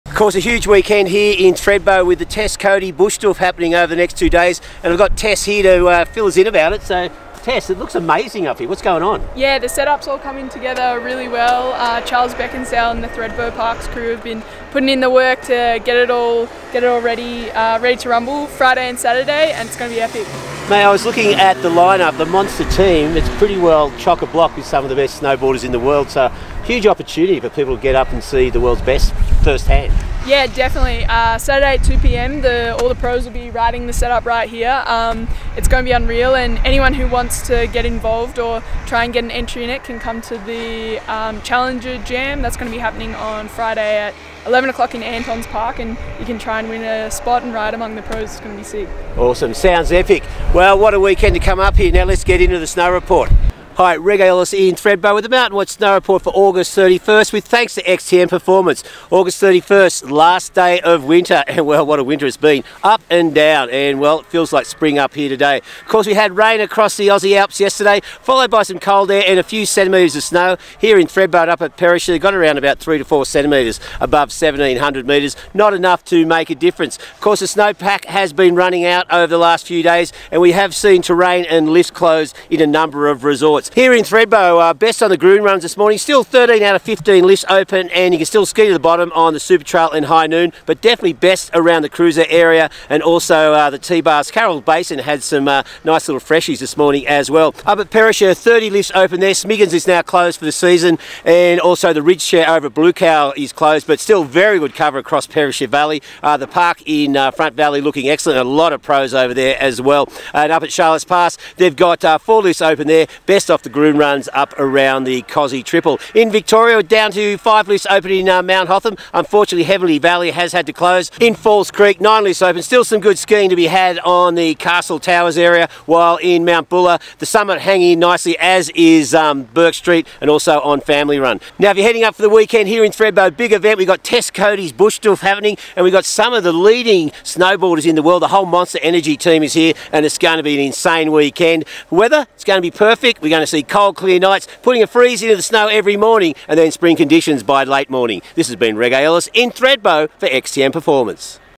Australian Snow Report | 31 August, 2023
It’s the last day of winter, Spring making an official appearance tomorrow 🌼☀️

Terrain is starting to thin out and close for the season across all resorts
It’s going to be a big weekend in Thredbo with the Tess Coady Bush Doof. The Monster Energy snowboard team are in town and its the perfect chance to see the pros riding your favourite local features.

@reggaeelliss has the latest